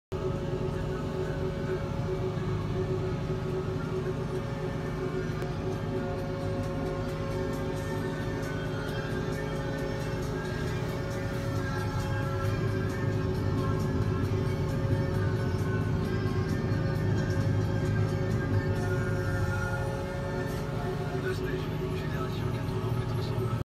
Labour fendt 716 Black et rabe 5 corps Claire voie
Labour nocturne avec fendt 716 s4 Black beauty et charrue rabe 5 corps NSH albatros .